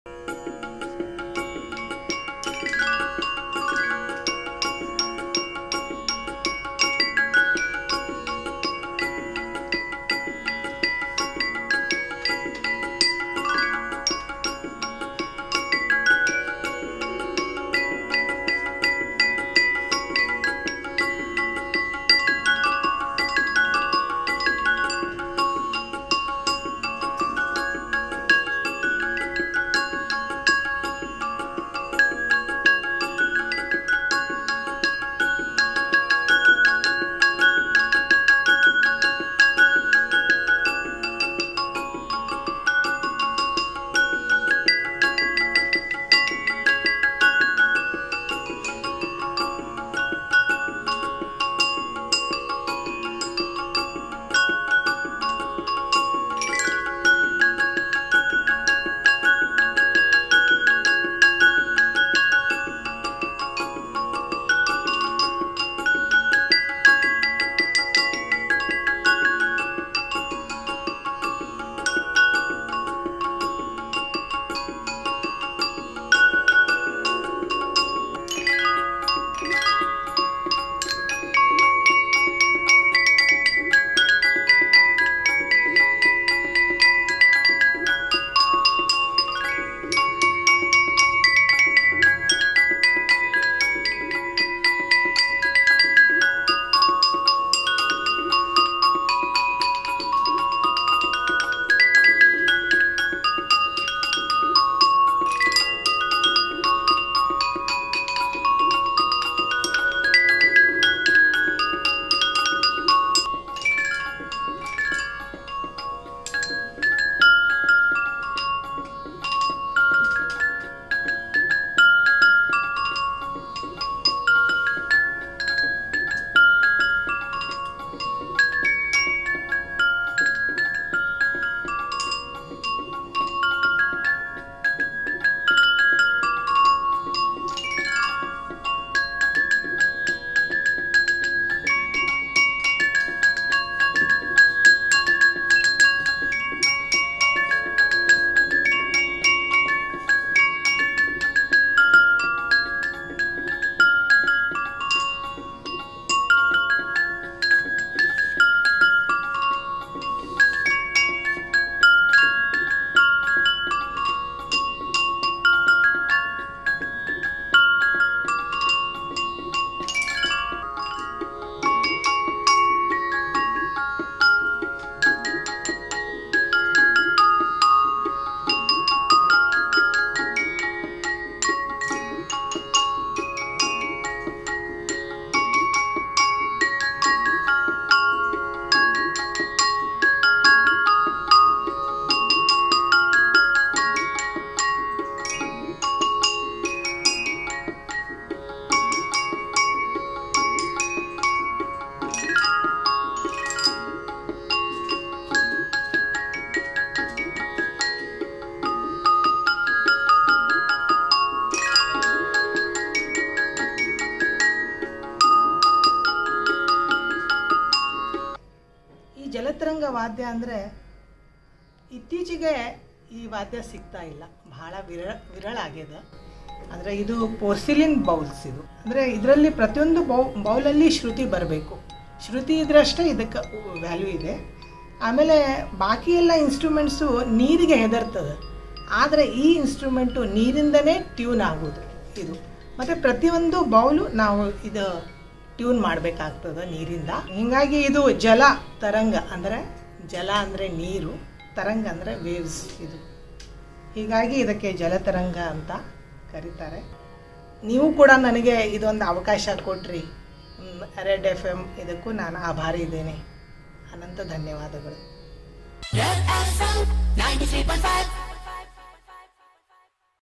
Jal tarang: The Rhythm of Water | ಇದು ನೀರಿನಿಂದ ಮೂಡಿಬಂದ ಸಂಗೀತ
ಅಪರೂಪದ ವಾದ್ಯವಾದಂತಹ ಜಲತರಂಗ ಉಳಿಸುತ್ತಿರುವ ವಿದೂಷಿ ಶಶಿಕಲಾ ದಾನಿ ಇವರು ನಮ್ಮ ಕರ್ನಾಟಕದ ಹೆಮ್ಮೆ.ಈ ಜಲತರಂಗದಲ್ಲಿ ನುಡಿಸಿದ ಹಾಡುಗಳನ್ನು ಗುರುತಿಸಿ ಕಾಮೆಂಟ್ ಮಾಡಿ.ಶೇರ್ ಮಾಡಿ.